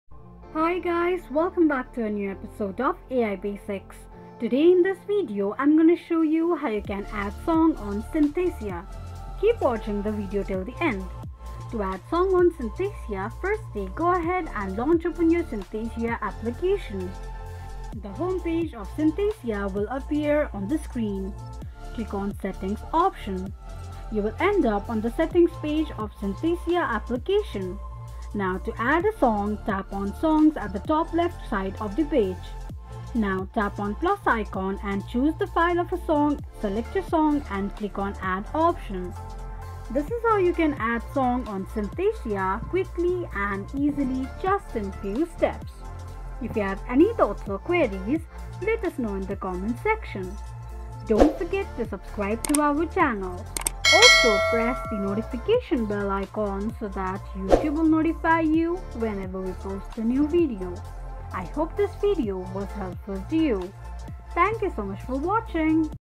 How to Add Song on Synthesia 2024 | Include Song on Synthesia
In today's video, we're focusing on a fundamental aspect of using Synthesia: adding songs to your repertoire. Whether you're a beginner or an experienced musician, knowing how to add songs to Synthesia is essential for expanding your musical skills and enjoyment.